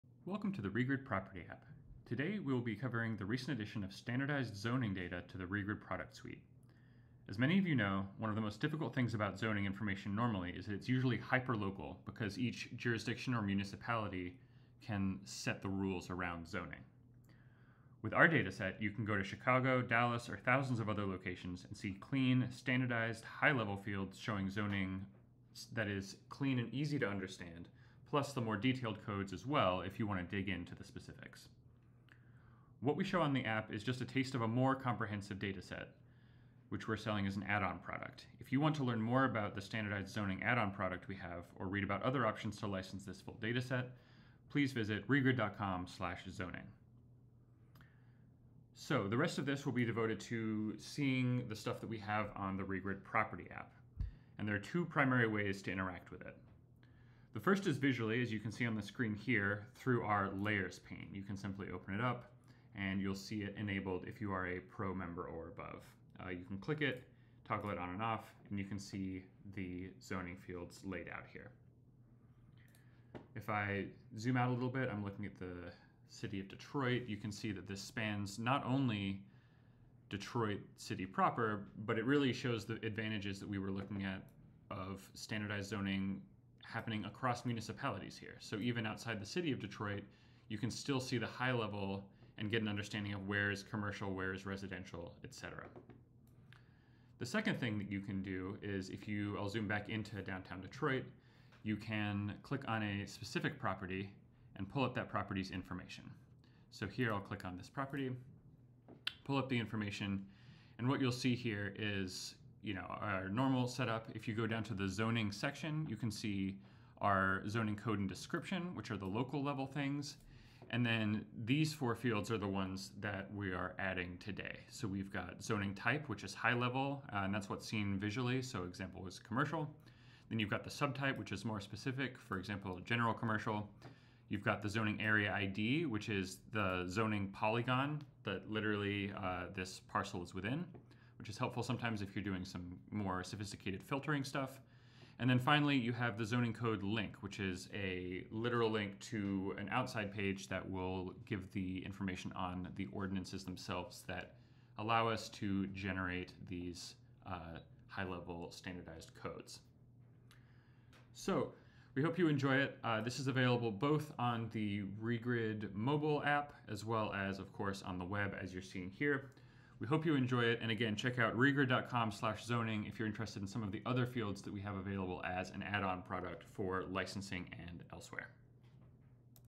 Standardized Zoning Fields - The Regrid Property App (Web)
Regrid now offers Standardized Zoning fields to help you easily understand zoning in 6,400 (and counting) municipalities nationwide. This video shows you how to access zoning's visual overlay via the Layers tab, as well as where the property-level zoning fields are in the "property details" section when clicked.

Note: the fields you see in the Regrid Property App are just a subset of our full Standardized Zoning Add-On product. Learn more about our full Standardized Zoning dataset